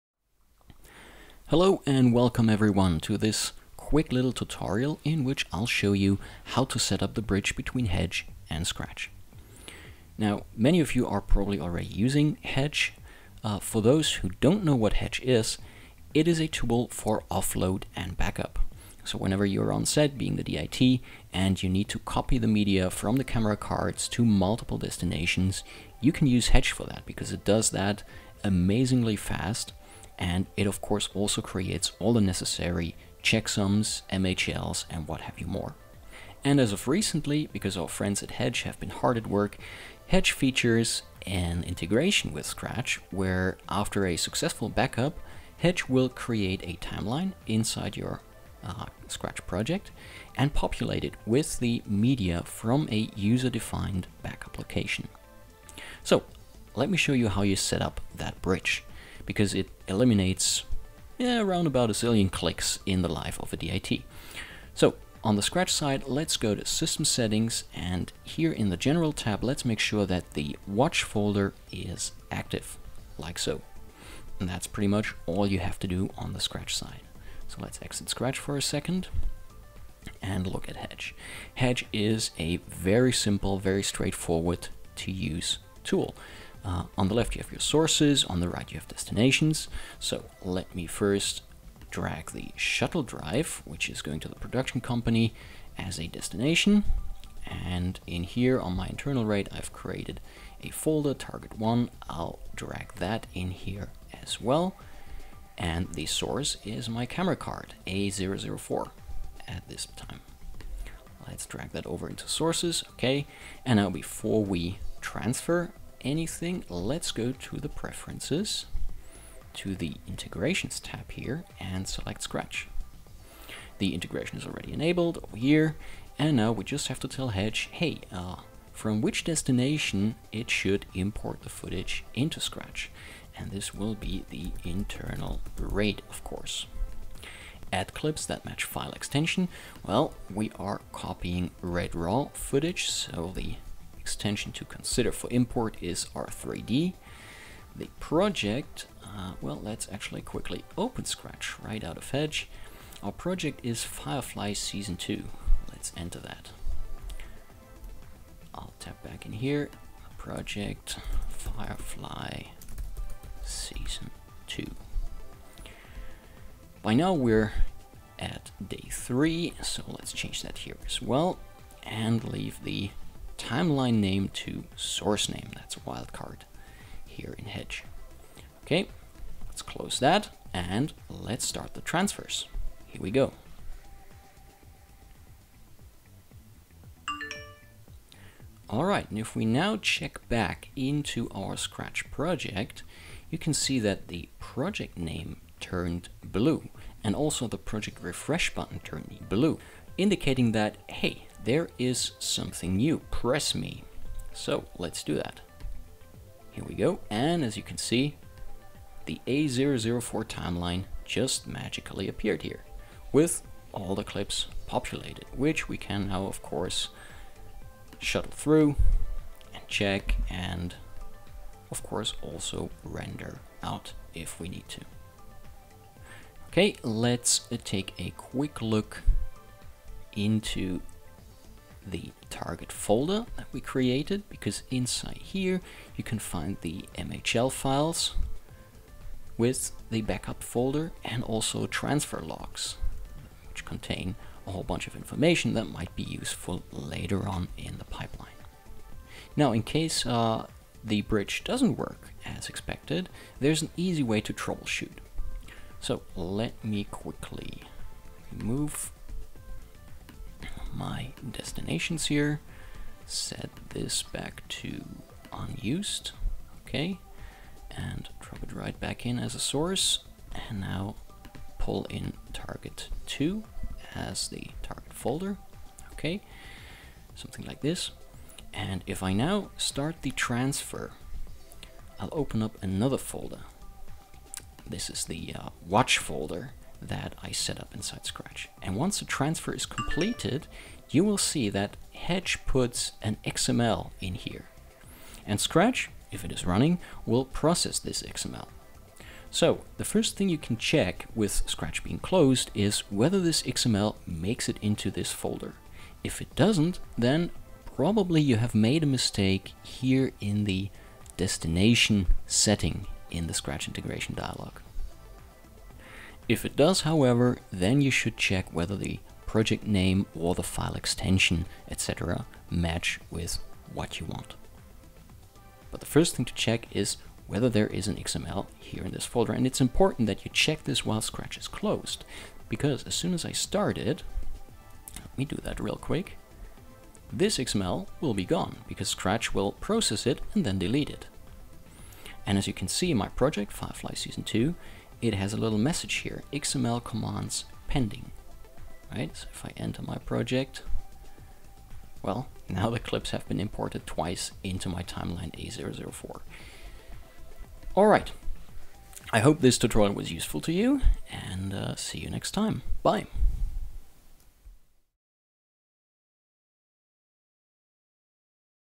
DIT Pack+: Hedge 2 SCRATCH
This video shows how to setup the bridge between Hedge and SCRATCH. Once set up, Hedge will automatically create a timeline inside the SCRATCH project and populate it with the backupped footage, saving roughly a zillion clicks for the DIT!